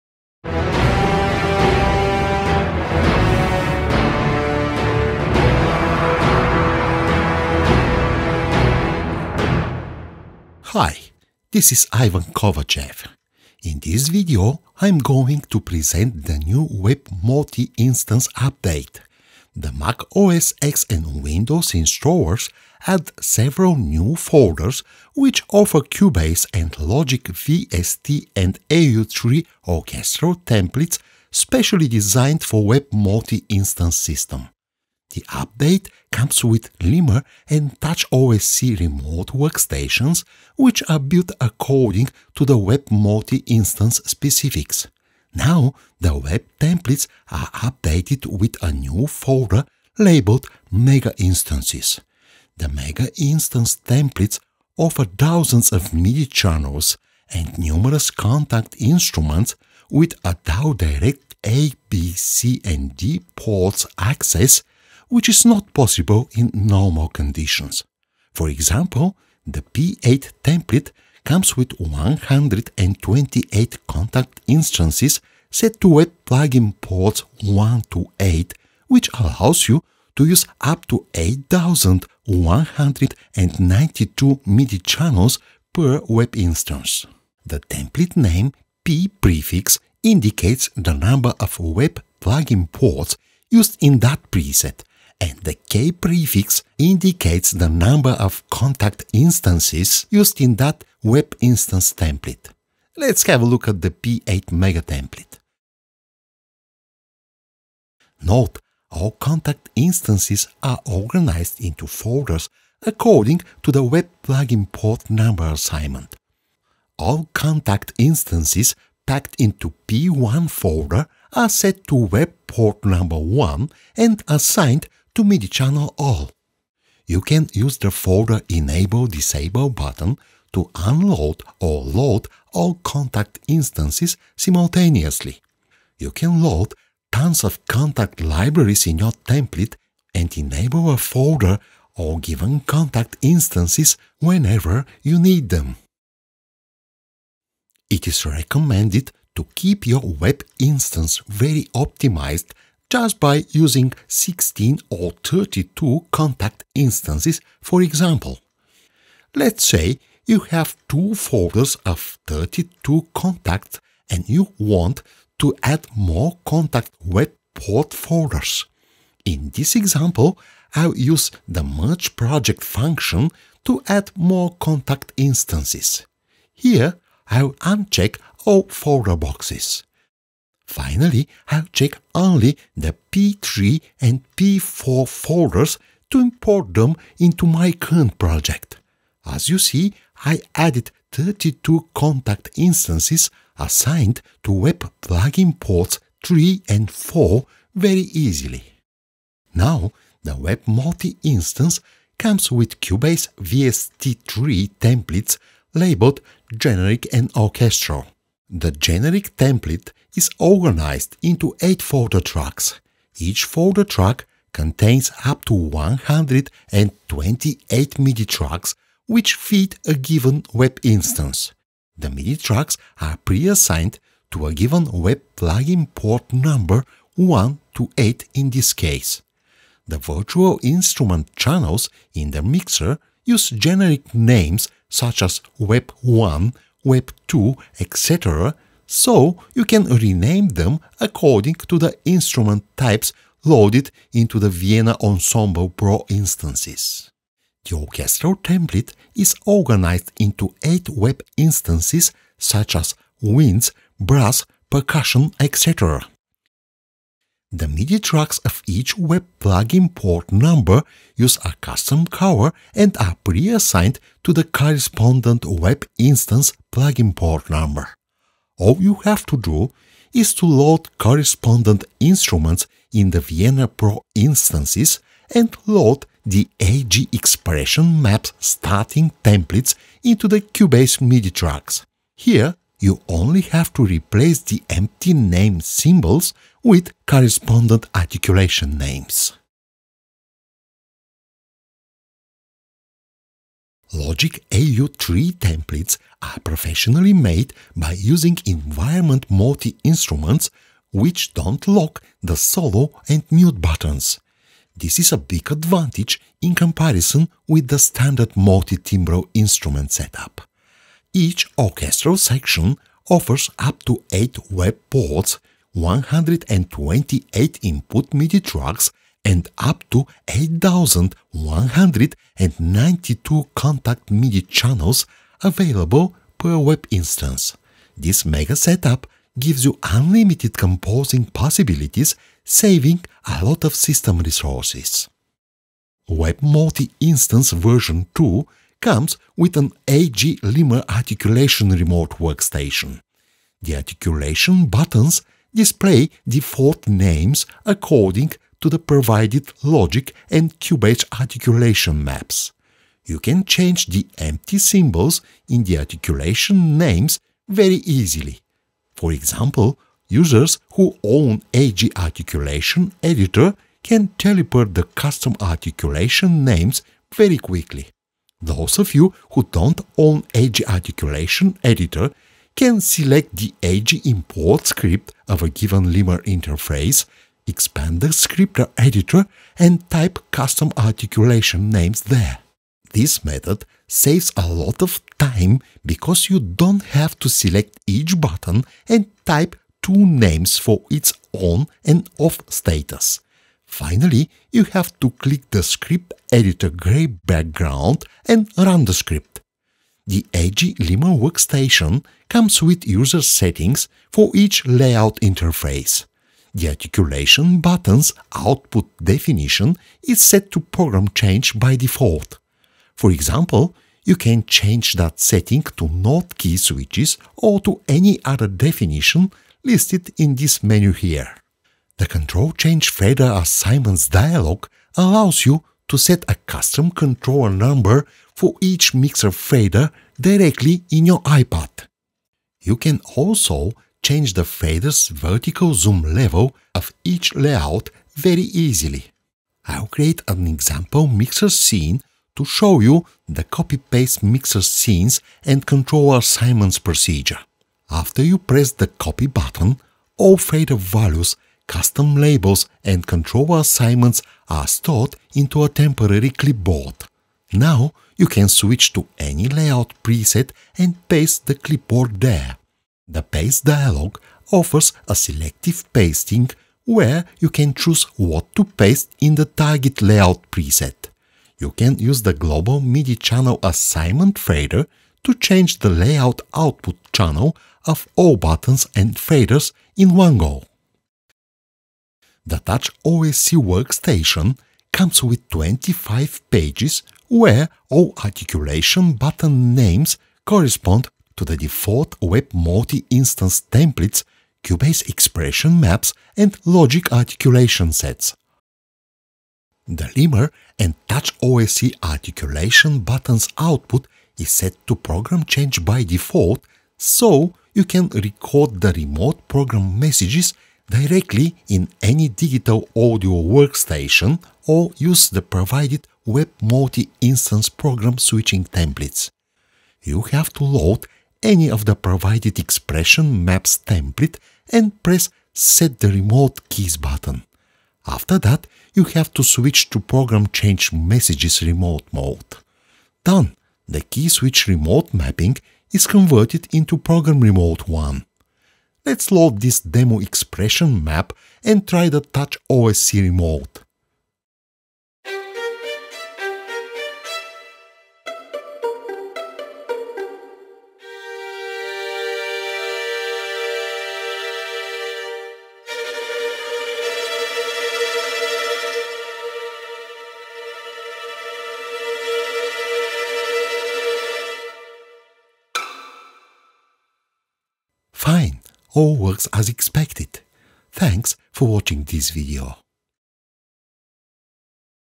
VEP MULTI INSTANCE v2.0 – Up to 128 Kontakts and 8192 Kontakt Channels per VEP Instance.
VEPM allows you to have a direct access to all
Kontakt ABCD Ports (64 MIDI Channels) directly from a single track in your DAW.
Now you can use up to 128 Kontakts and up to 8192 Kontakt channels per VEP Instance.
VEP MULTI saves a lot of Kontakt instances usage (1:4), as well as CPU & GPU resources.